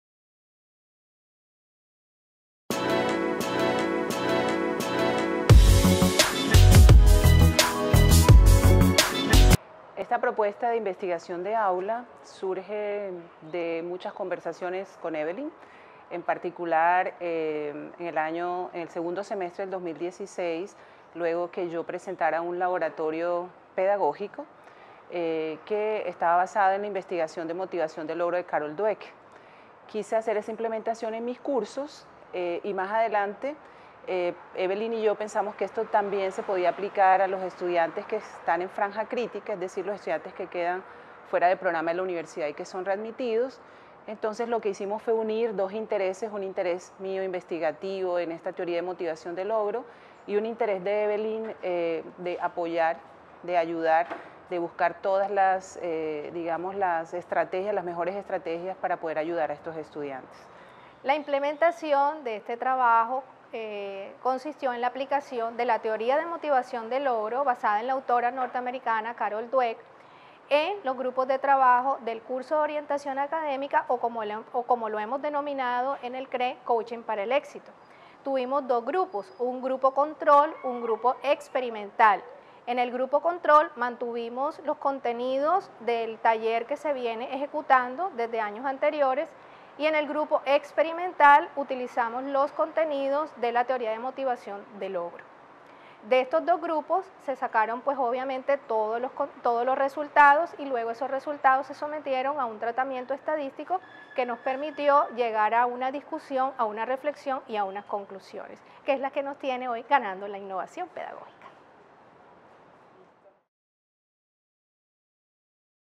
Mayilin Moreno y Evelyn Llinás, ganadoras del Concurso de Innovación Pedagógica 2018
Mayilin Moreno, del departamento de Psicología, y Evelyn Llinás, coordinadora de orientación academica del CREE, hablan sobre su propuesta en Investigaión de Aula y su experiencia en el Concurso de Innovación Pedagógica 2018.